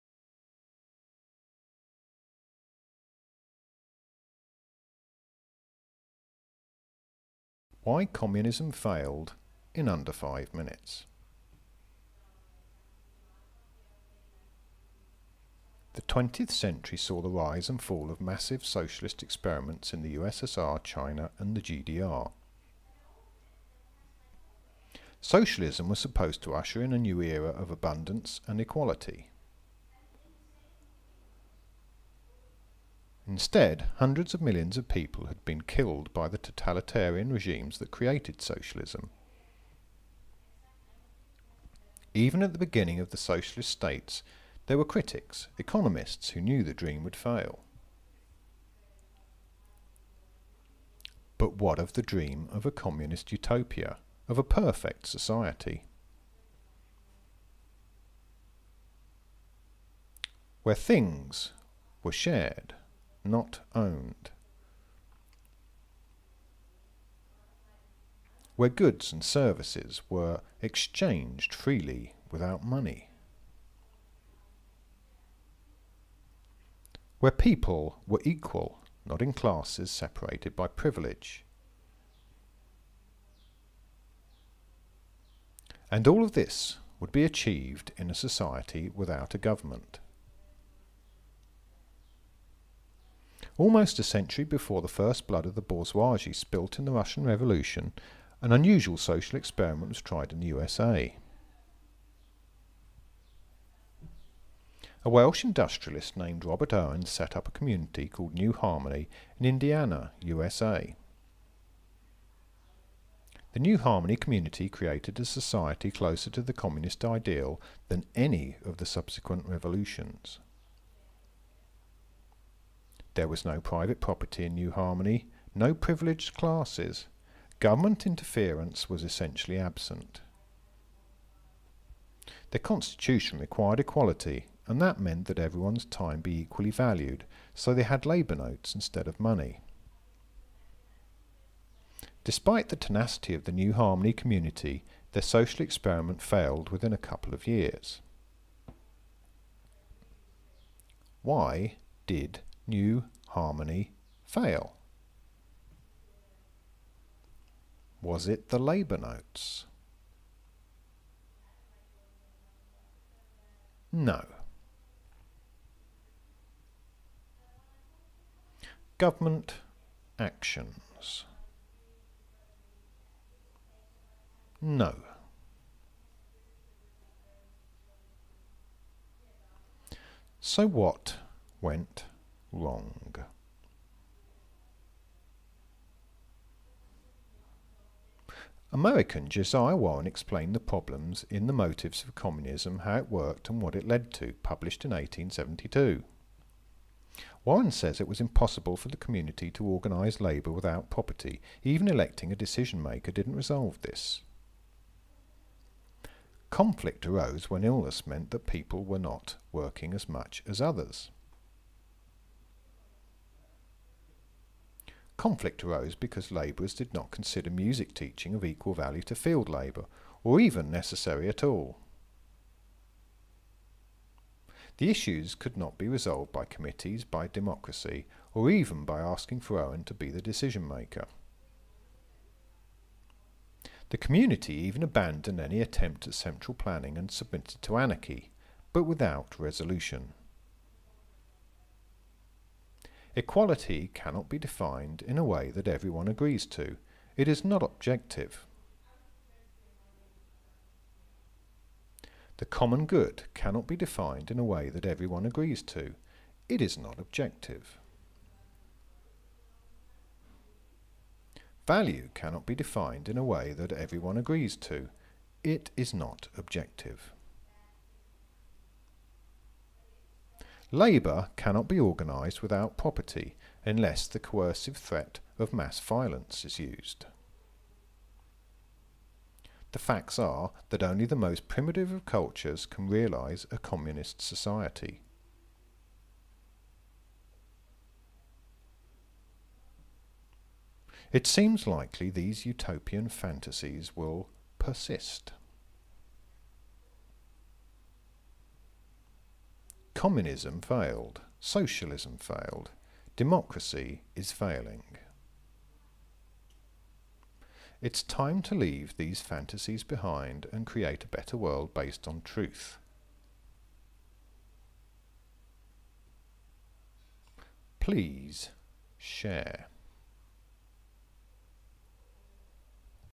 FP037 Why Communism Failed in under 5 minutes
This presentation gives the reasons why the communist societies established by utopian socialists fail based on the real life experiences of the New Harmony community that was established in Indiana in 1825 by Robert Owen.

No experiment is perfect, least of all a social experiment. The New Harmony community seemed to have sincerely attempted to create a community based on grass roots socialist principles of the day. They implement to a greater or lesser extent each of the criteria of a  communist society. The community used "labour notes", rather than entirely free exchange, regular currency being prohibited.

Unlike the anarchist societies that emerged during conflicts in Europe and the Ukraine, the New Harmony experiment was not destroyed by reactionary forces. It imploded due to irreconcilable differences of opinion because the foundational principles of.their society were not objective.

The New Harmony Community was one of a number of socialist projects aimed at creating a communist utopia, according to Wikipedia:
"Between 1825 and 1830 more than a dozen such colonies were established in the United States, inspired by the ideas of Robert Owen. All of these met with economic failure and rapid disestablishment within one or a comparatively few years."

Sources:
Communist society (defined)
"In political and social sciences, communism (from Latin communis – common, universal) is a social, political, and economic ideology and movement whose ultimate goal is the establishment of the communist society, which is a socioeconomic order structured upon the common ownership of the means of production, absence of social classes, money, and the state."

List of Owenite communities in the United States (they all failed with a year or so)